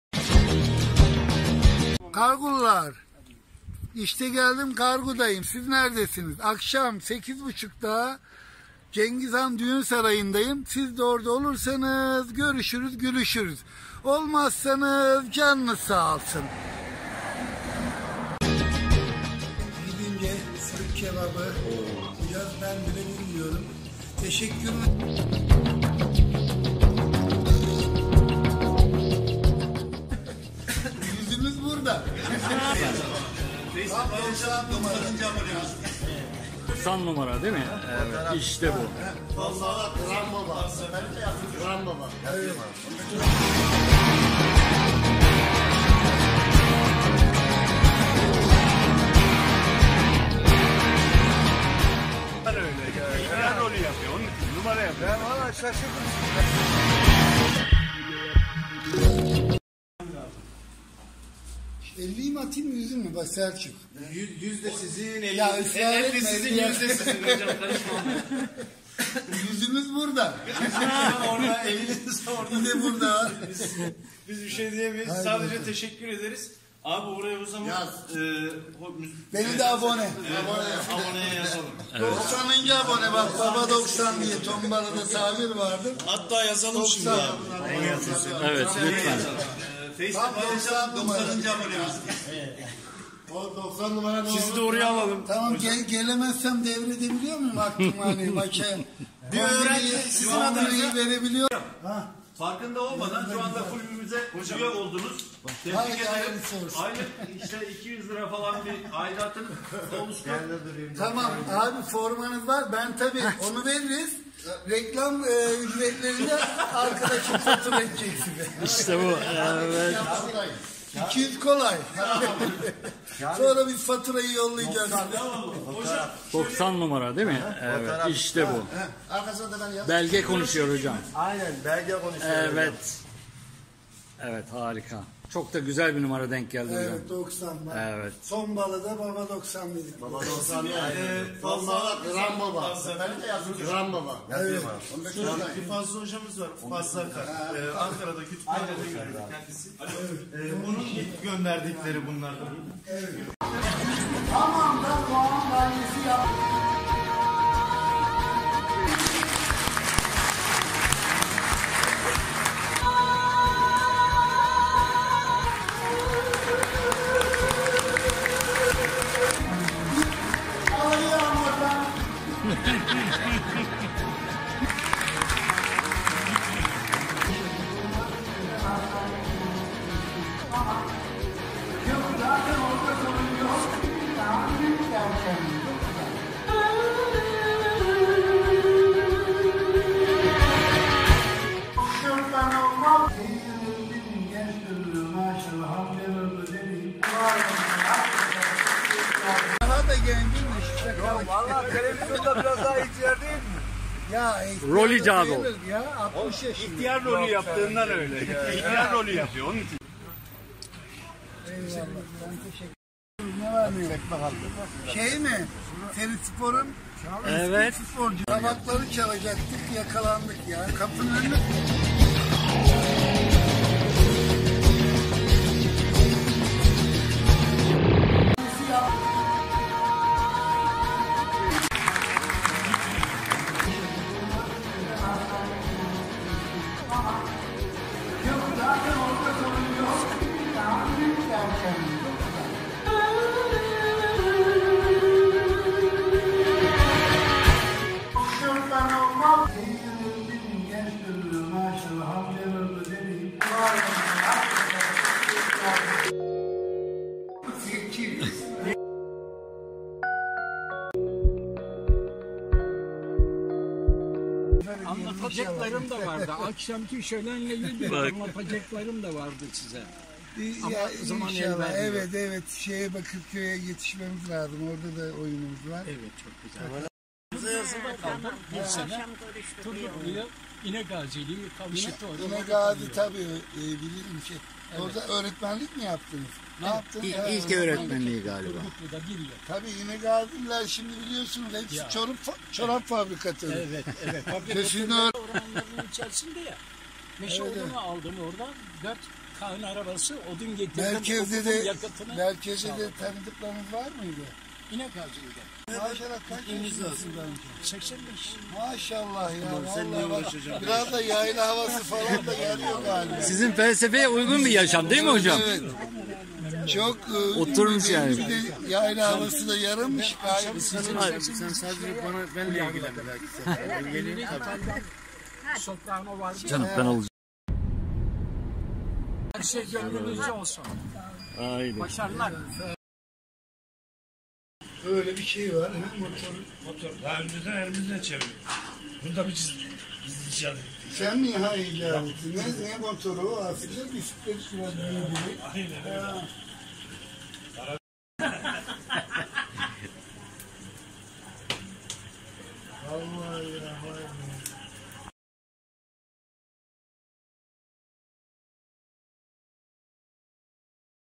Kargı Serispor Mandıra Filozofunu ağırladı...
Kargı halkıyla buluşan Mandıra Filozofu Müfit Can Saçıntı hemşehrileriyle kucaklaştı...